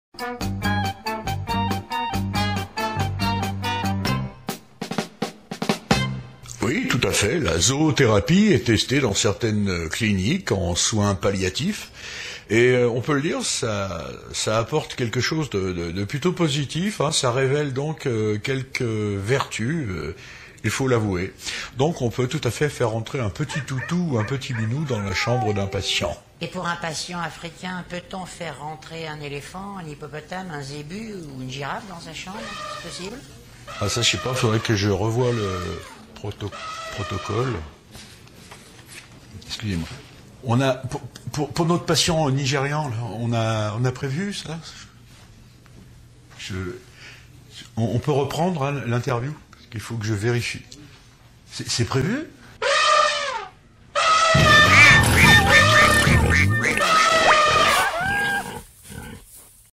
Les effets positifs de la zoothérapie
La zoothérapie aurait des effets positifs sur notre santé.
Témoignage d'un professionnel de la santé ;-)